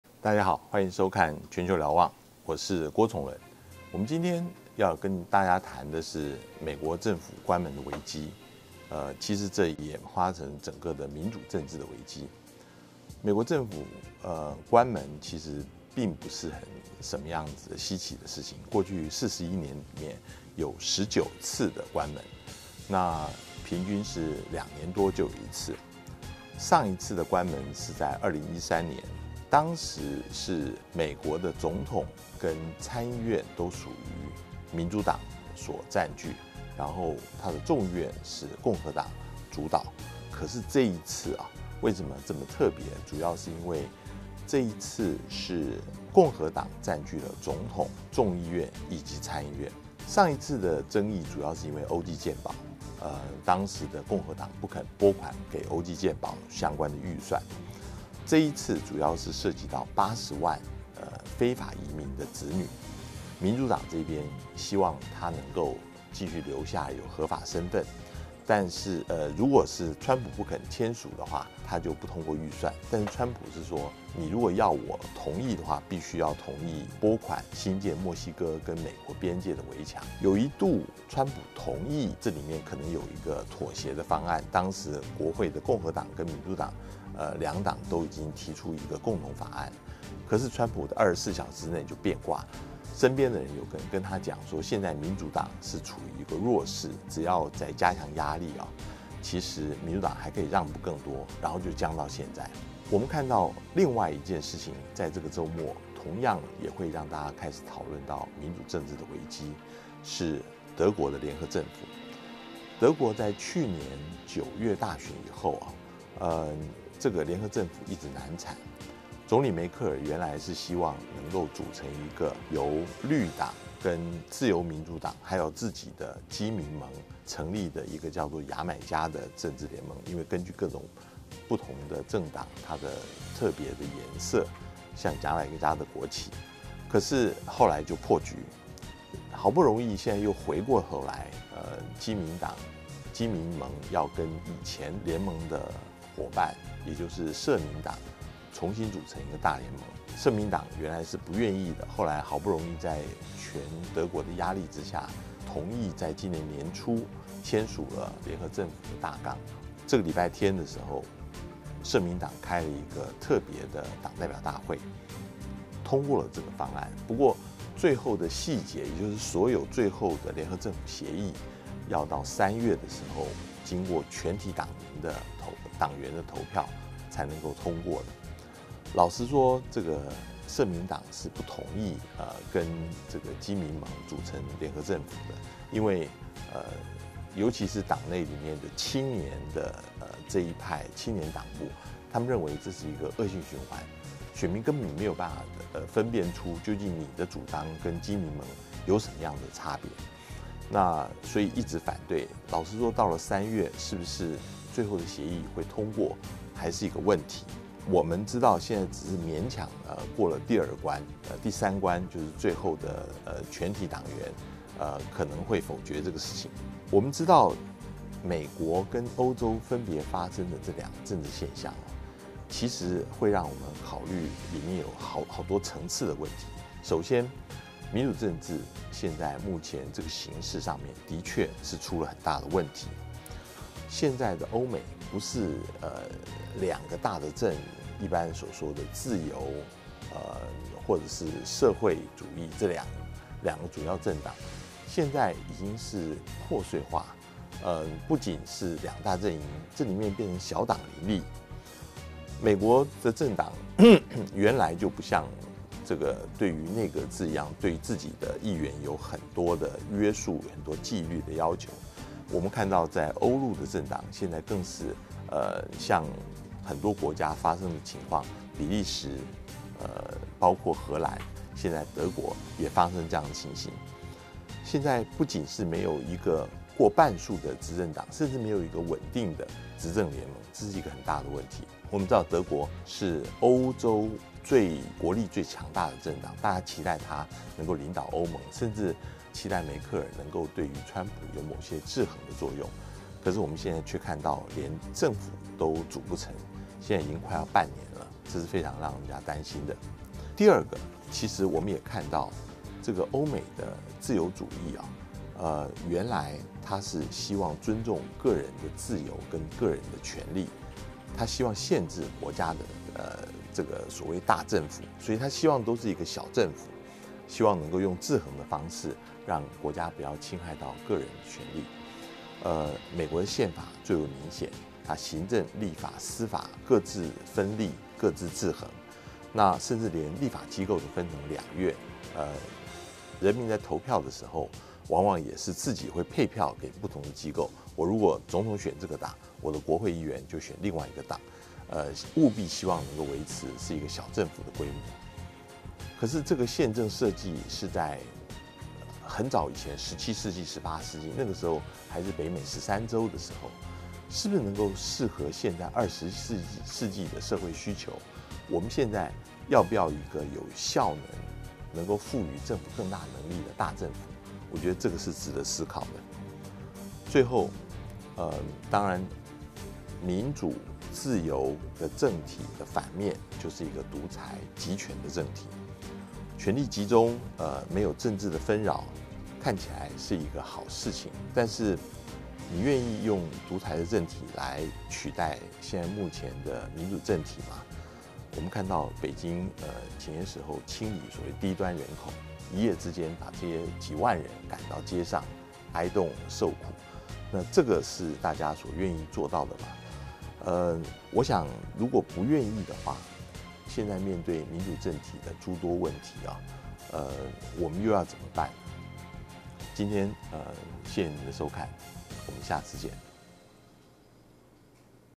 0122 民主政體危機 郭崇倫深入剖析
美國聯邦政府關門危機、德國近半年組不成聯合政府，觀察美歐的這兩個政治現象，除了觀其來龍去脈，聽聽郭崇倫從更宏觀的角度來看民主政體危機。